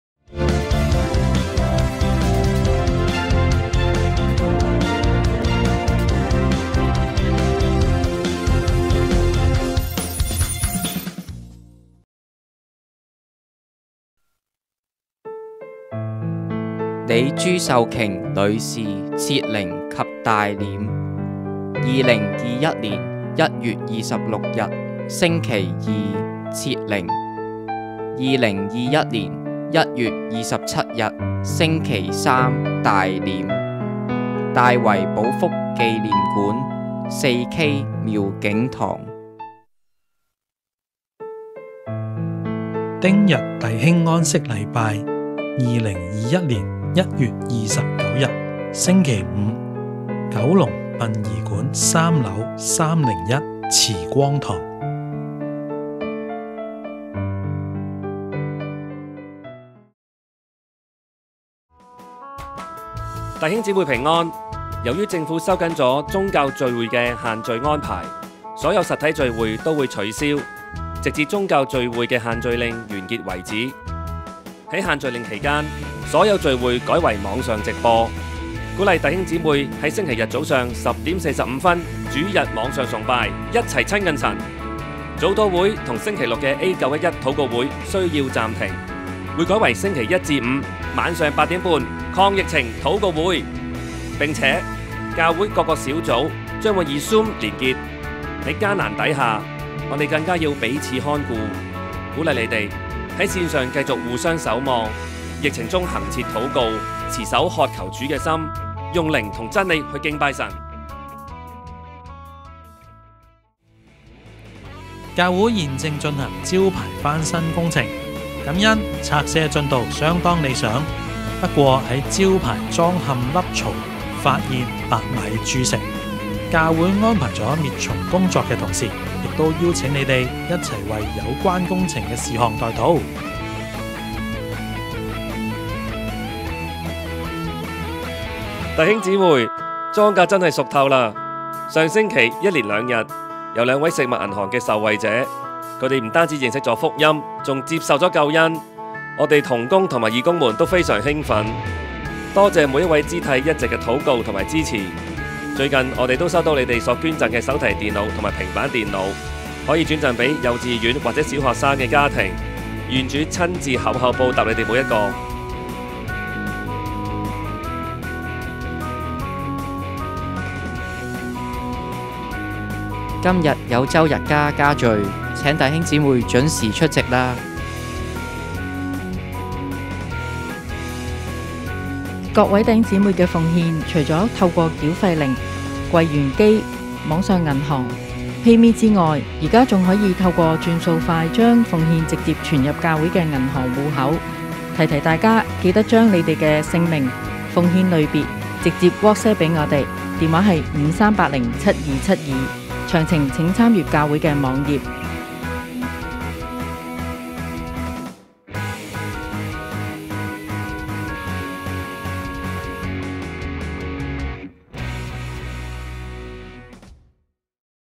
2021年1月17日 復興家事
1/ 李朱秀琼女士設靈及大殮（週五家沙龍組Anna李淑薇姊妹之先母）

    丁日弟兄安息禮拜（週日家泉源組Doris丁麗芬之先父）

2/ 限聚令期間網上聚會指引

3/ 教會招牌工程進行中，請禱告記念

4/ 關愛活動回顧

5/ 週日家家聚
Zoom及YouTube直播
日期：2021年1月17日
時間：下午1點

6/ 網上奉獻安排